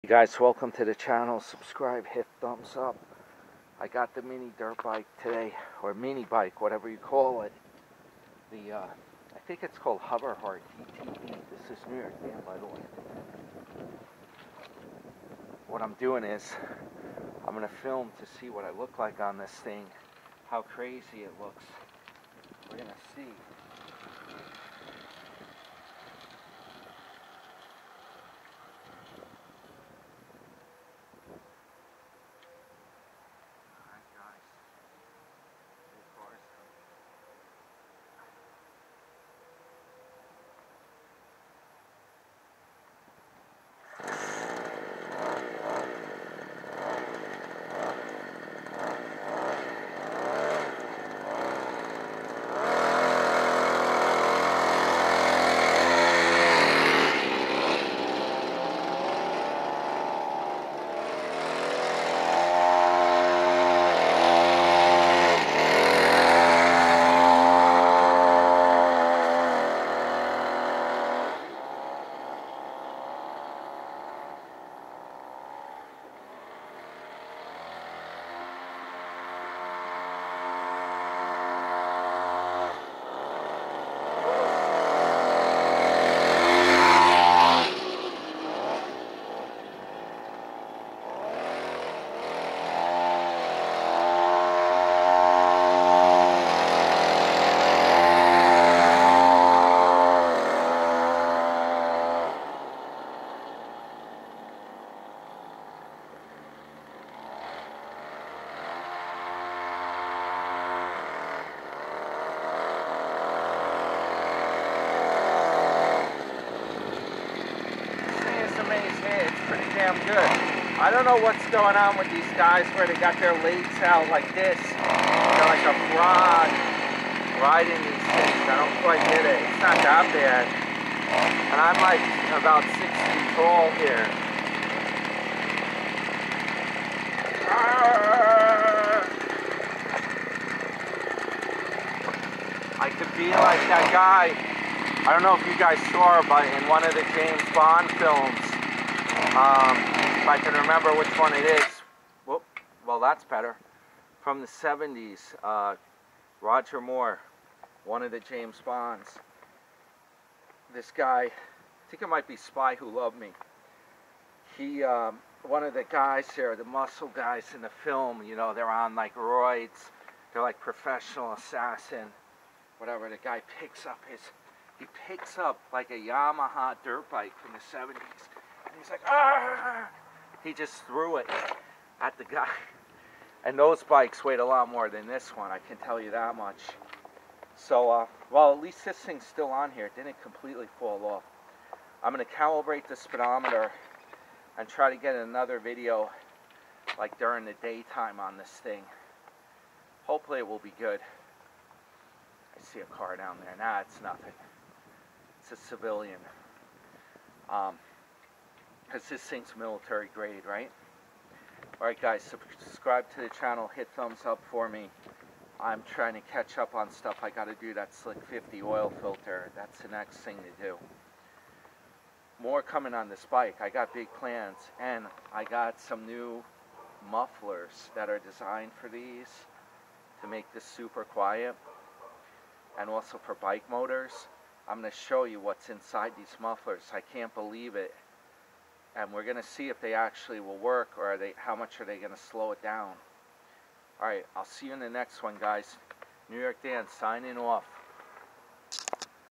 $250 Walmart Minibike Hover Heart Brand - Me Riding Outside View - Subscribe - Full Video Next!
Hit Thumbs Up To Spread The Motercycle Joy!